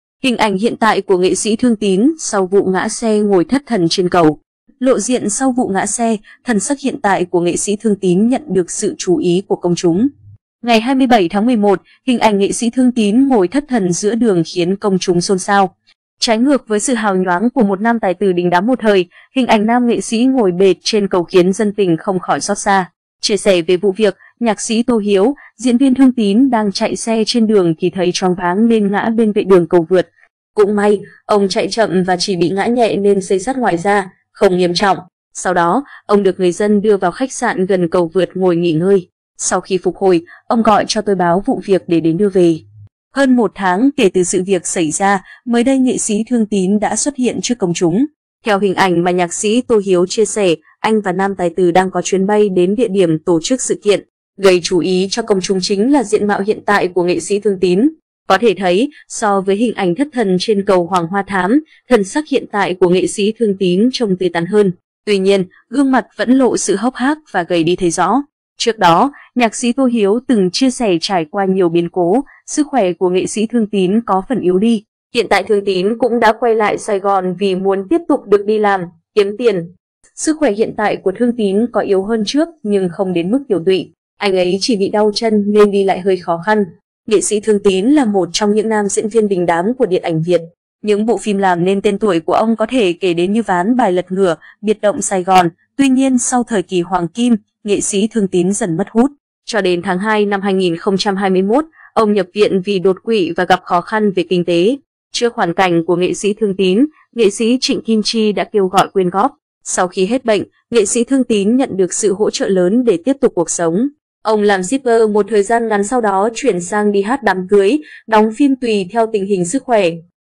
Hình ảnh hiện tại của nghệ sĩ Thương Tín sau vụ ngã xe ngồi thất thần trên cầu
Lộ diện sau vụ ngã xe, thần sắc hiện tại của NS Thương Tín nhận được sự chú ý của công chúng.Trước đó, nhạc sĩ Tô Hiếu từng chia sẻ trải qua nhiều biến cố, sức khỏe của NS Thương Tín có phần yếu đi. "Hiện tại Thương Tín cũng đã quay lại Sài Gòn vì muốn tiếp tục được đi làm, kiếm tiền. Sức khỏe hiện tại của Thương Tín có yếu hơn trước nhưng không đến mức tiều tụy. Anh ấy chỉ bị đau chân nên đi lại hơi khó khăn".
Ngày 27/11, hình ảnh NS Thương Tín ngồi thất thần giữa đường khiến công chúng xôn xao. Trái ngược với sự hào nhoáng của một nam tài tử đình đám một thời, hình ảnh nam nghệ sĩ ngồi bệt trên cầu khiến dân tình không khỏi xót xa.
Chia sẻ về vụ việc, nhạc sĩ Tô Hiếu: "Diễn viên Thương Tín đang chạy xe trên đường thì thấy choáng váng nên ngã bên vệ đường cầu vượt. Cũng may, ông chạy chậm và chỉ bị ngã nhẹ nên xây xát ngoài da, không nghiêm trọng. Sau đó, ông được người dân đưa vào khách sạn gần cầu vượt ngồi nghỉ ngơi. Sau khi phục hồi, ông gọi cho tôi báo vụ việc để đến đưa về".
Có thể thấy, so với hình ảnh thất thần trên cầu Hoàng Hoa Thám, thần sắc hiện tại của NS Thương Tín trông tươi tắn hơn. Tuy nhiên, gương mặt vẫn lộ sự hốc hác và gầy đi thấy rõ.
Hình ảnh NS Thương Tín ngồi thất thần từng khiến dân tình xôn xao
NS Thương Tín là một trong những nam diễn viên đình đám của điện ảnh Việt. Những bộ phim làm nên tên tuổi của ông có thể kể đến như "Ván bài lật ngửa", "Biệt động Sài Gòn",... Tuy nhiên sau thời kỳ hoàng kim, NS Thương Tín dần mất hút.
Cho đến tháng 2/2021, ông nhập viện vì đột quỵ và gặp khó khăn về kinh tế. Trước hoàn cảnh của nghệ sĩ Thương Tín, nghệ sĩ Trịnh Kim Chi đã kêu gọi quyên góp. Sau khi hết bệnh, nghệ sĩ Thương Tín nhận được sự hỗ trợ lớn để tiếp tục cuộc sống. Ông làm shipper một thời gian ngắn sau đó chuyển sang đi hát đám cưới, đóng phim tùy theo tình hình sức khỏe.
Hơn 1 tháng kể từ sự việc xảy ra, mới đây NS Thương Tín đã xuất hiện trước công chúng. Theo hình ảnh mà nhạc sĩ Tô Hiếu chia sẻ, anh và nam tài tử đang có chuyến bay đến địa điểm tổ chức sự kiện. Gây chú ý cho công chúng chính là diện mạo hiện tại của NS Thương Tín.
Hình ảnh hiện tại của NS Thương Tín